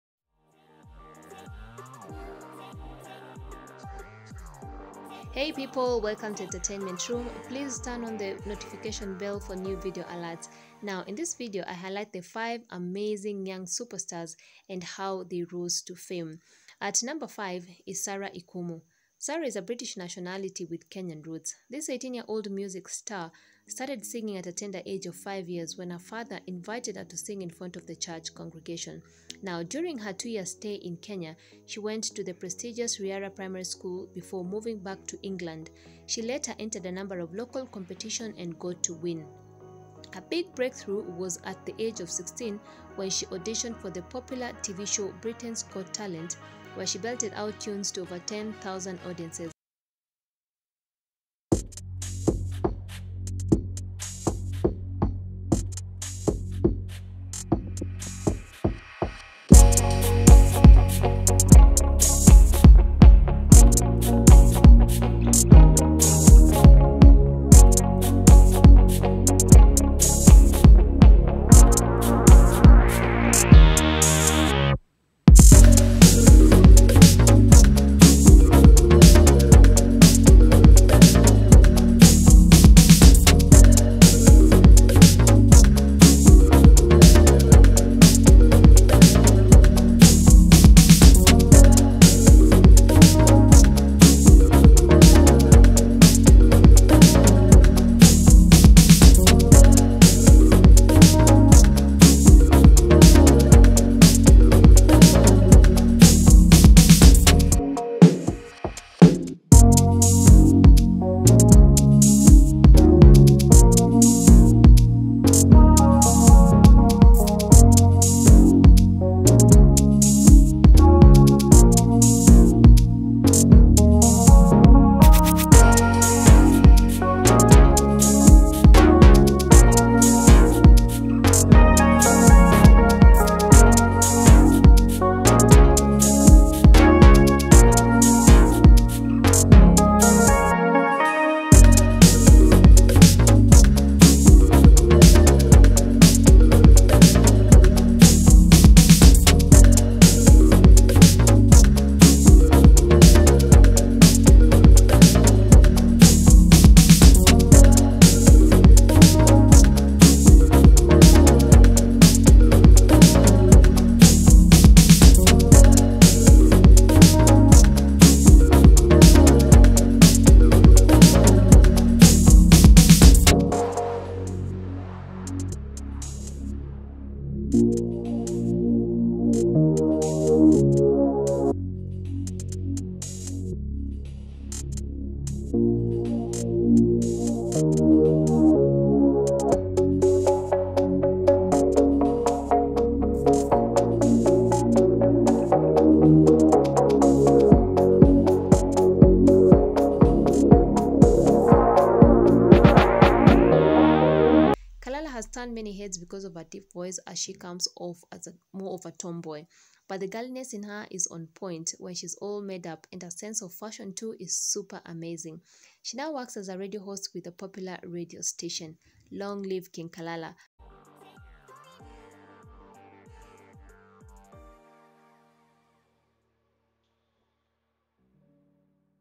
YOUNG amazing SUPERSTARS | Here is how they rose to fame | A must WATCH, you will LOVE them.
Here are the top five amazing young superstars and how they rose to fame. enjoy the video.